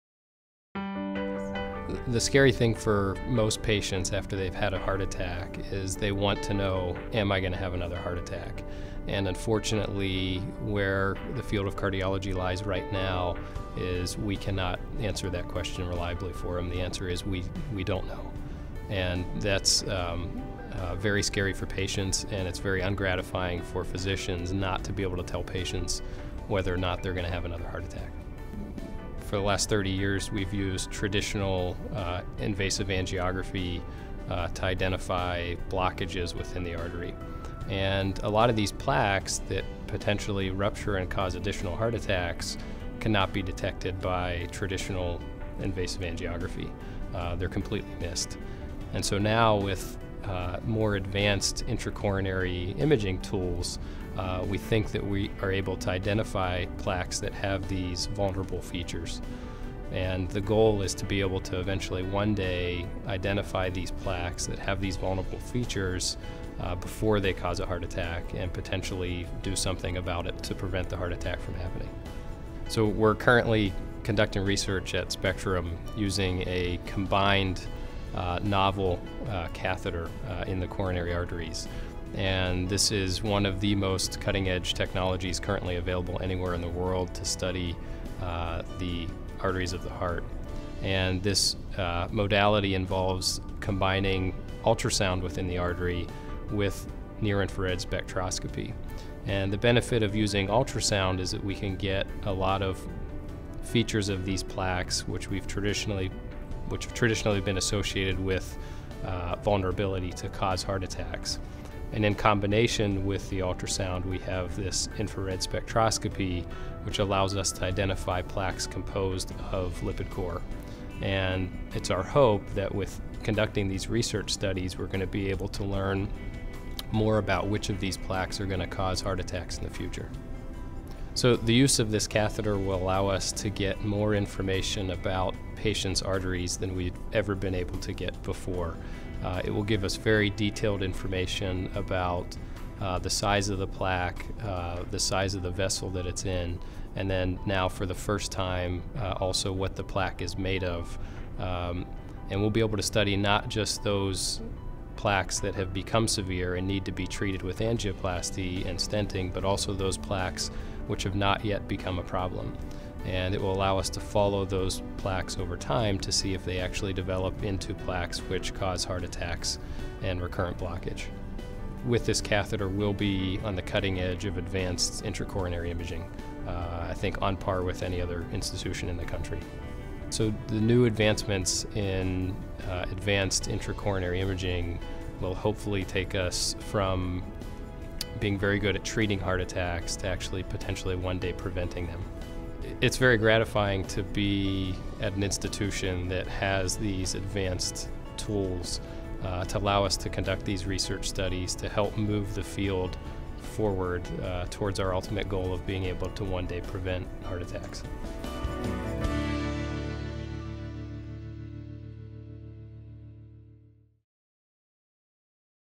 Clinical Research to Predict and Prevent Heart Attacks - Ryan Madder, MD
Dr. Madder describes a new device under investigation that may be able to determine if the plaque in a patient's arteries may put them at risk for future heart attacks.